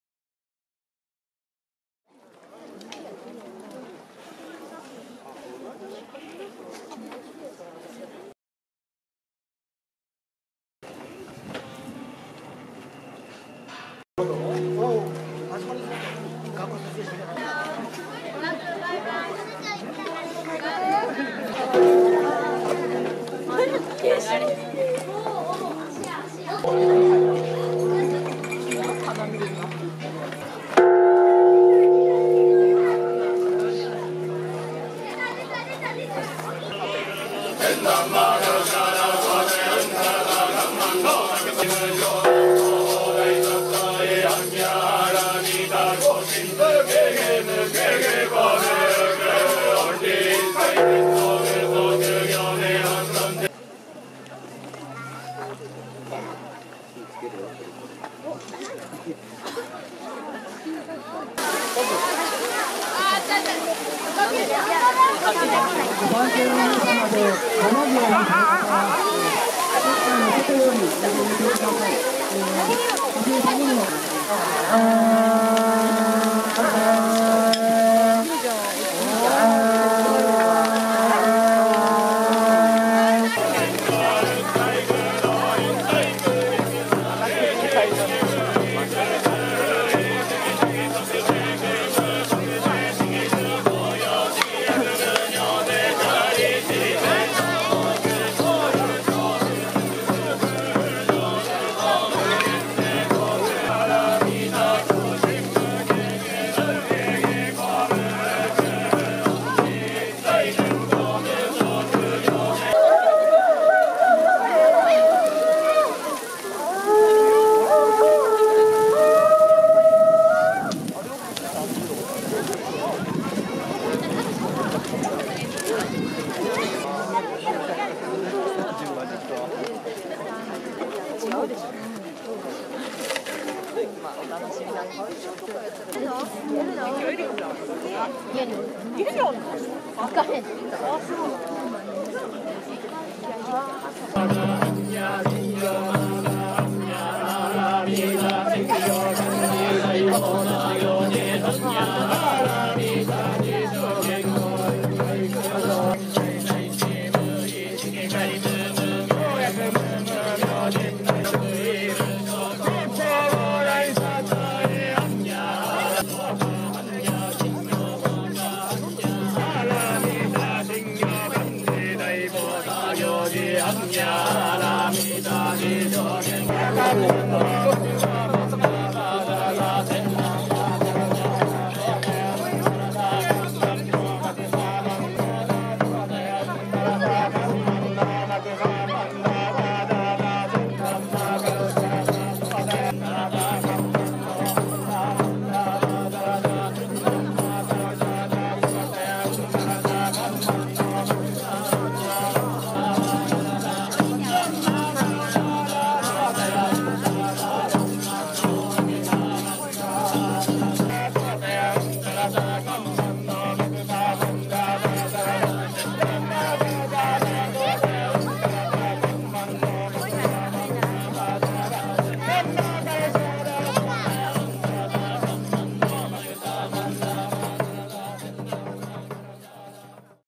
2015 金生山名星輪寺 火渡り DMC-FZ1000 4K
岐阜県大垣市赤坂にある金生山名星輪寺の初こくぞうの様子です。

初めて火渡りを体験しました。丸太の間から炎が上っていましたが、意外と熱さは感じませんでした。厄落としに是非☆